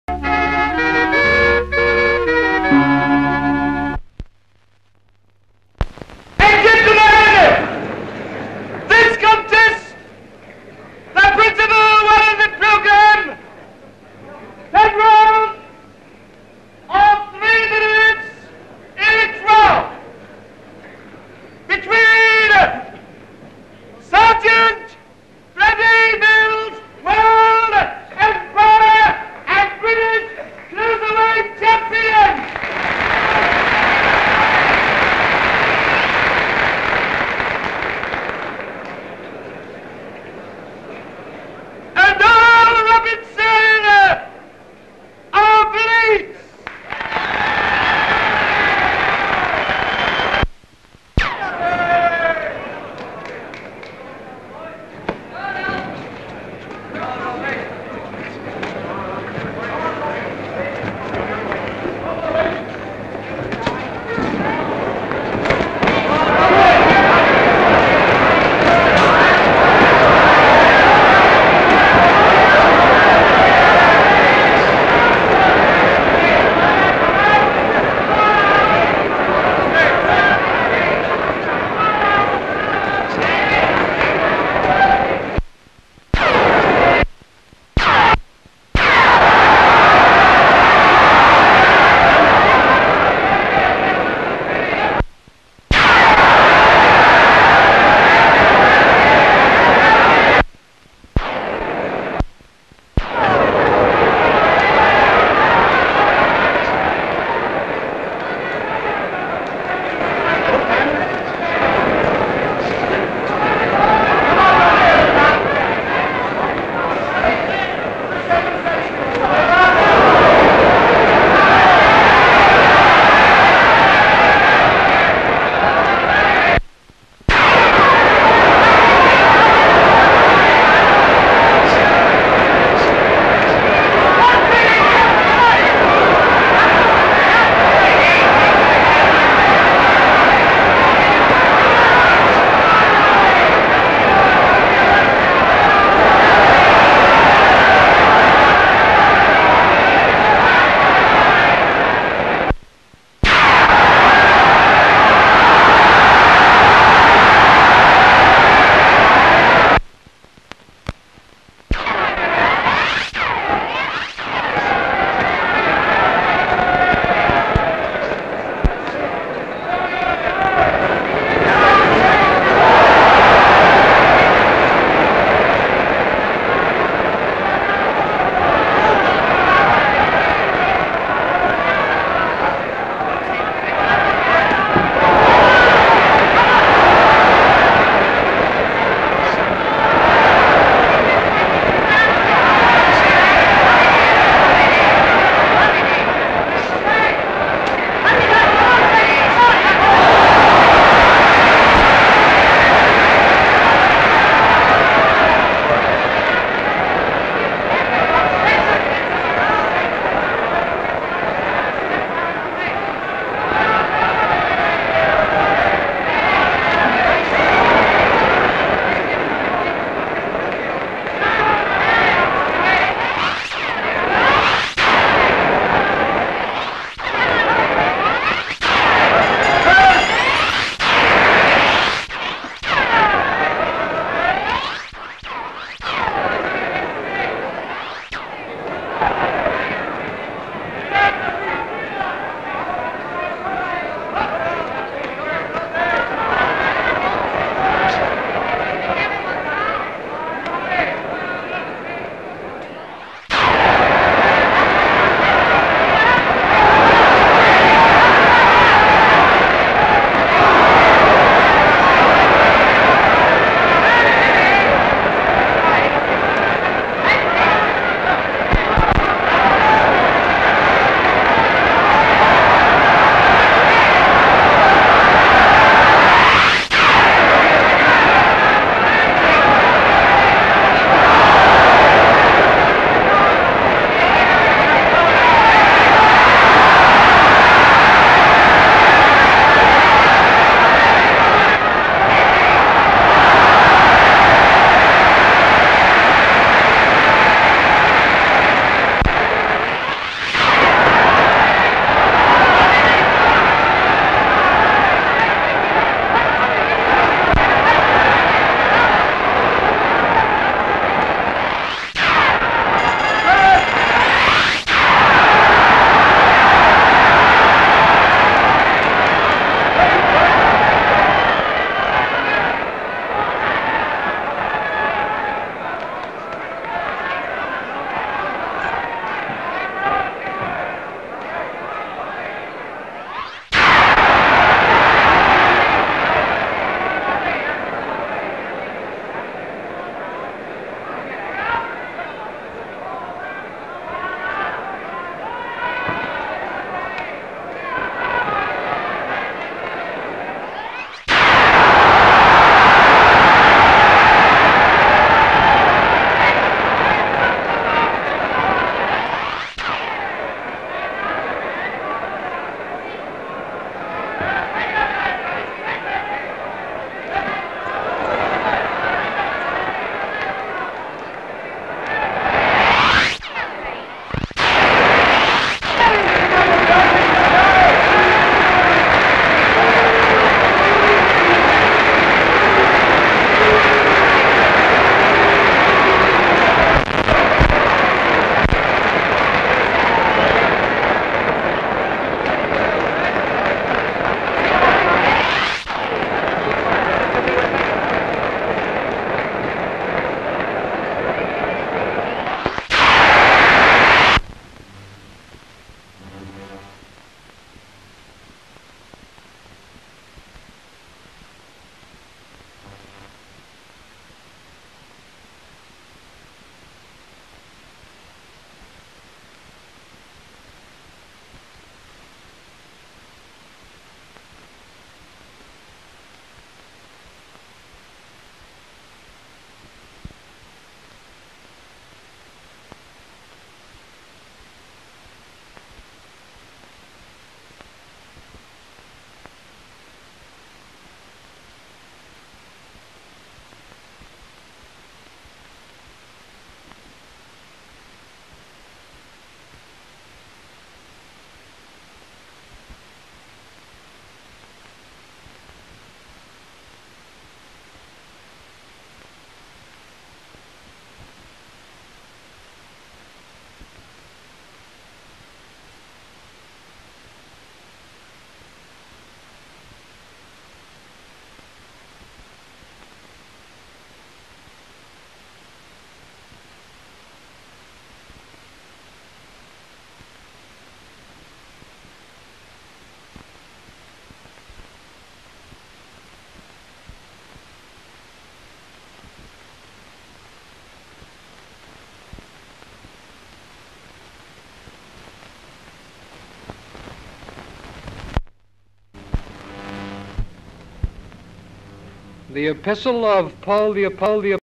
FREDDIE MILLS V AND SARGEANT ROBINSON - SOUND
Shot of ring, with referee announcing boxers, who each in turn jumps up & acknowledges same. Various shots of match, rather a dull fight. Referee announcing Mills as winner Mills & Robinson shaking hands at end. Shot of crowds. CU. Two shaking hands. Mills leaving ring. Robinson leaving ring amid cheers.